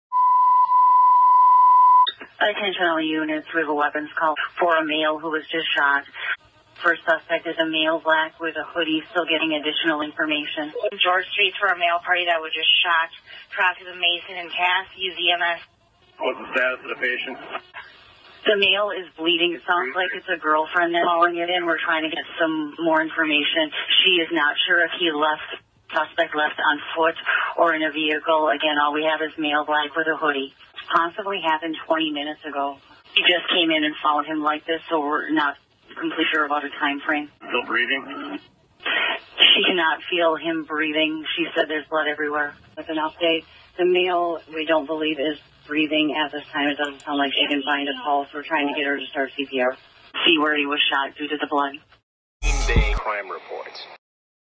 George St homicide 1-27-19
7:22 pm 1/27/19
600 block George St (east GB) - WEAPONS CALL - male party was shot - rescue will stage - suspect is a male/black who left in unknown direction

(audio provided by Broadcastify)